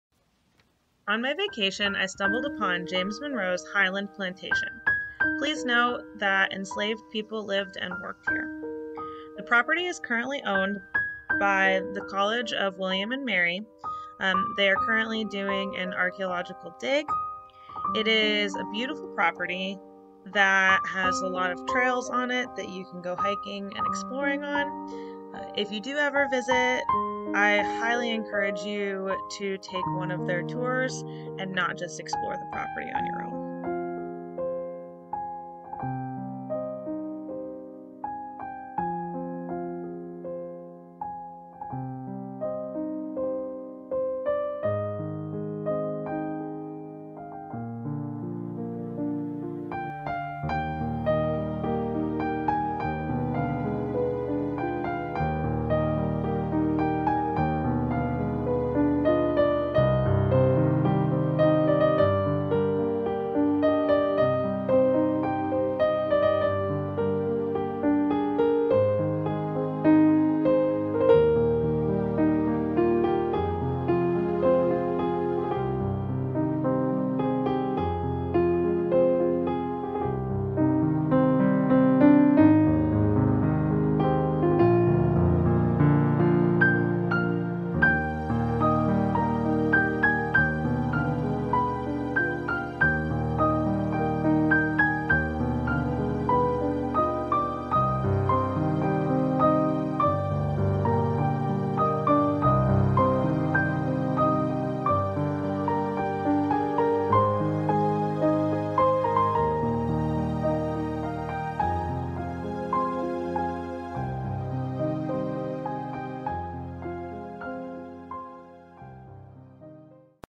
James Monroe's Highland
On my vacation I visited James Monroe's Highland.